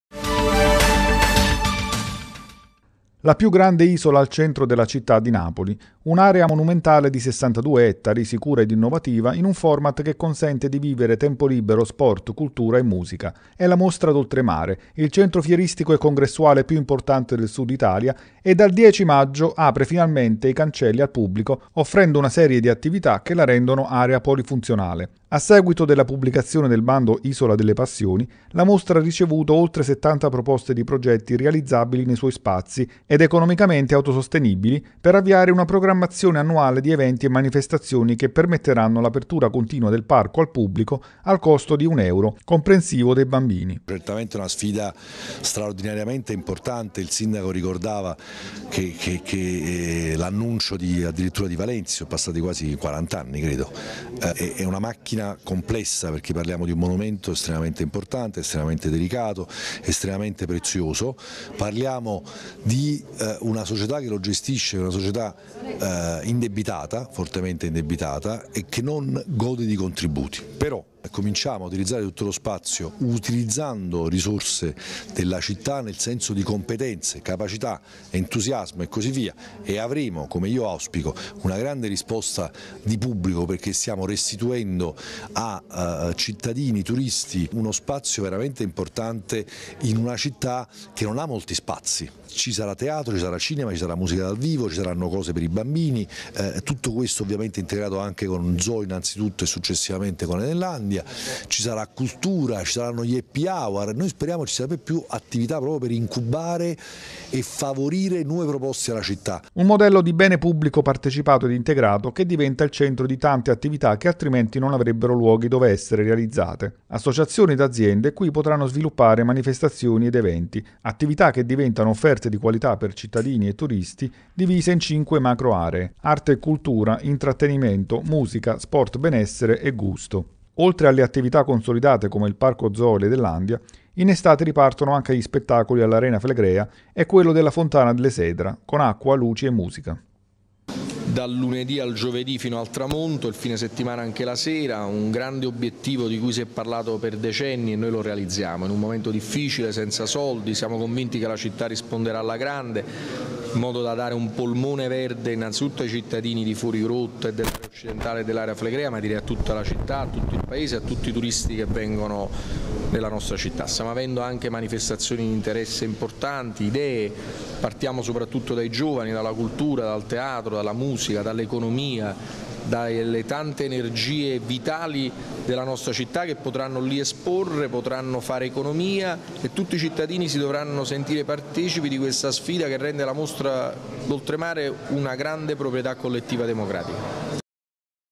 La Mostra d'Oltremare apre le porte alla città
Dal 10 maggio, il più grande polo fieristico e monumentale del Sud resterà aperto, pieno di iniziative ed eventi.